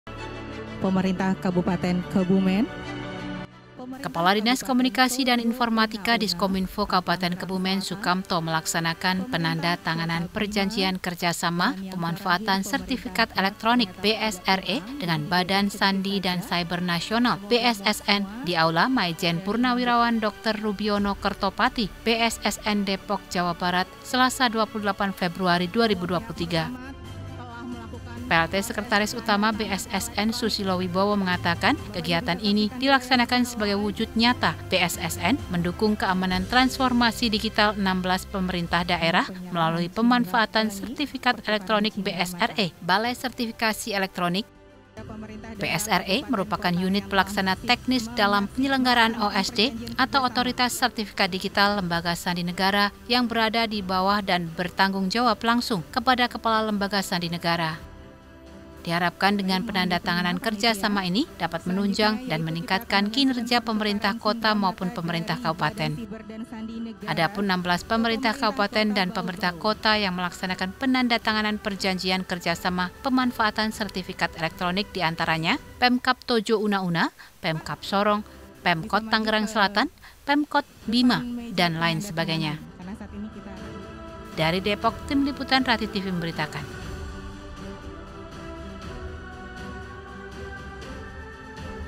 Kebumen Tanda Tangani Perjanjian Kerja Sama dengan BSSN
Berita Kebumen Edisi Selasa, 28 Februari 2023

Kepala Dinas Komunikasi dan Informatika Kabupaten Kebumen ikut mendatangani perjanjian kerja sama pemanfaatan sertifikat elektronik dengan Badan Siber dan Sandi Negara atau BSSN. Perjanjian kerja sama dilakukan untuk mendukung transformasi digital dan peningkatan layanan pemerintah berbasis elektronik.